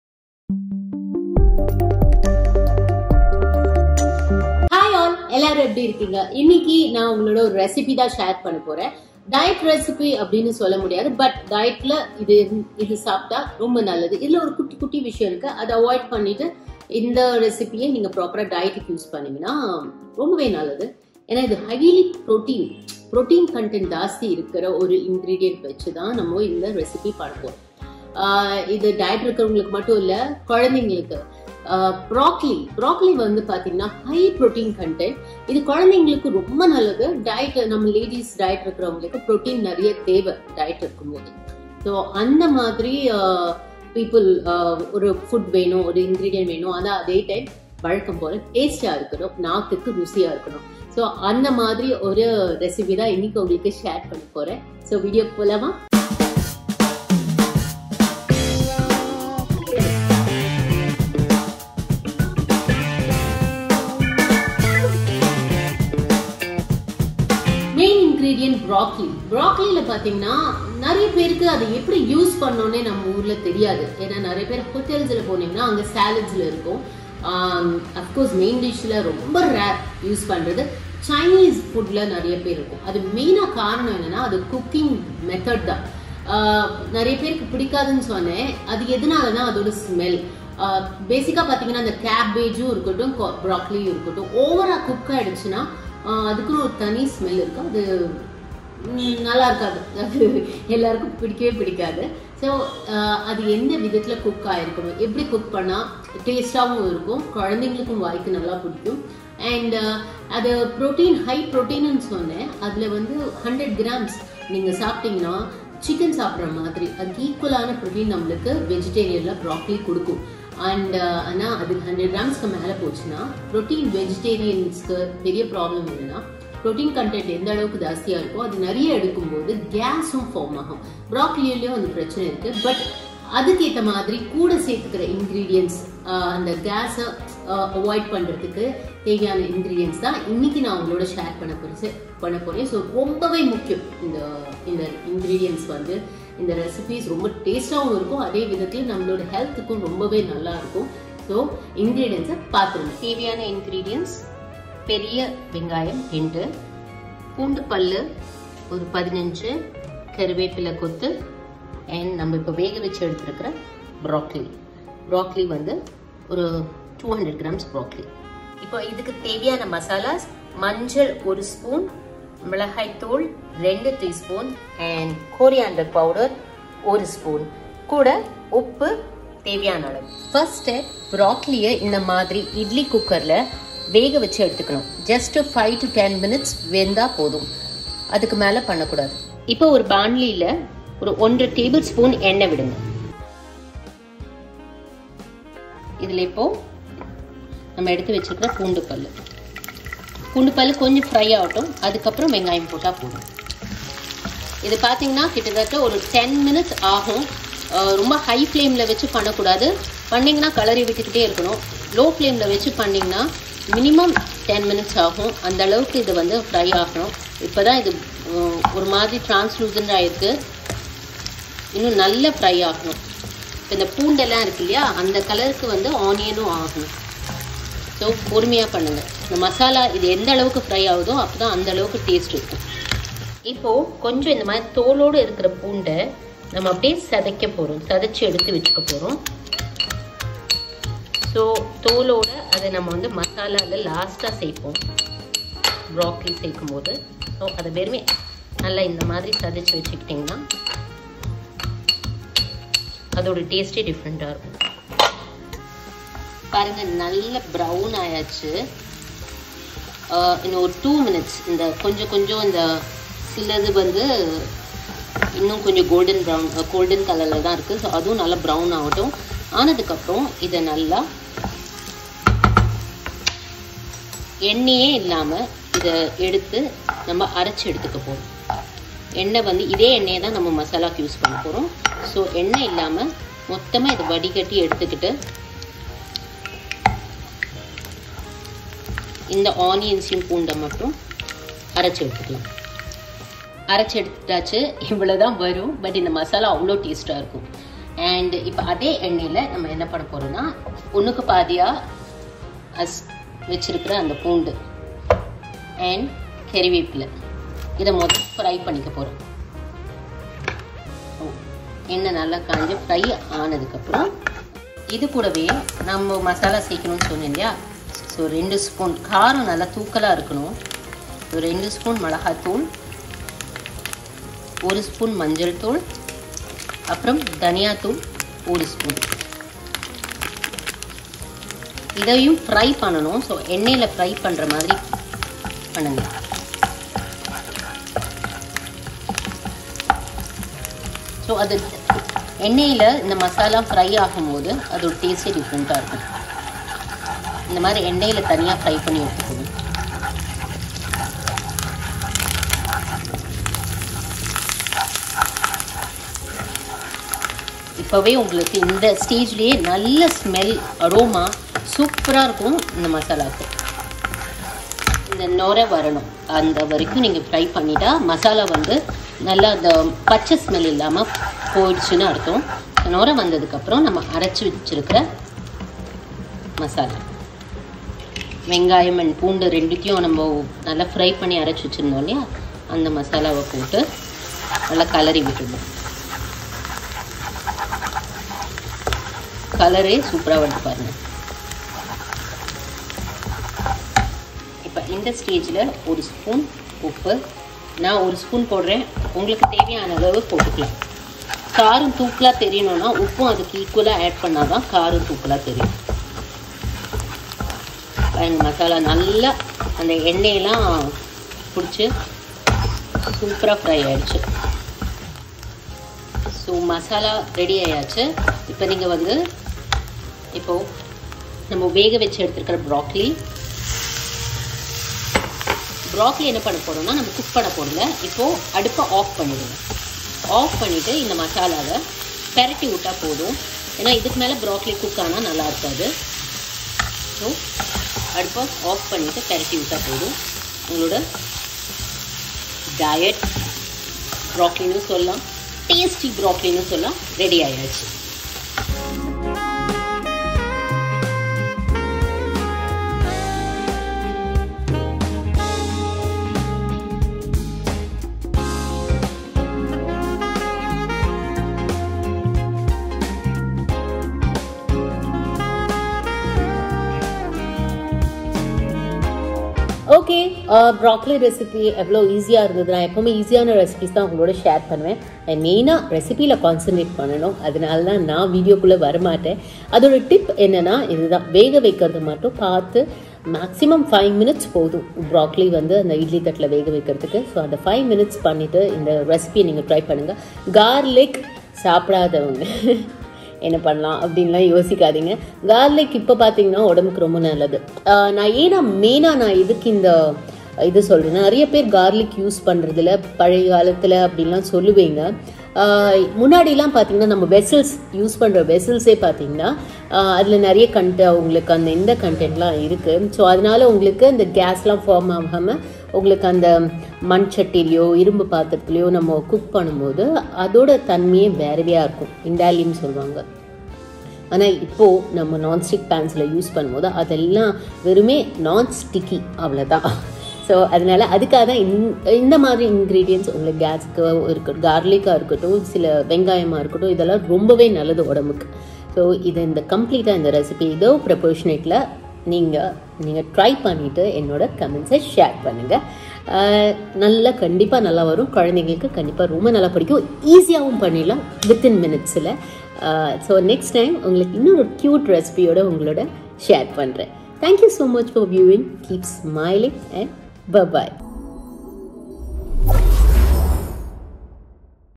Easy Diet Recipe | Simple Breakfast Idea | Broccoli Spl | Preethi Sanjiv
Easy Diet Recipe and a Simple Breakfast Idea is shared in this Broccoli Special Vlog of Preethi Sanjiv!

More updates and videos from the shooting spot awaits for you. .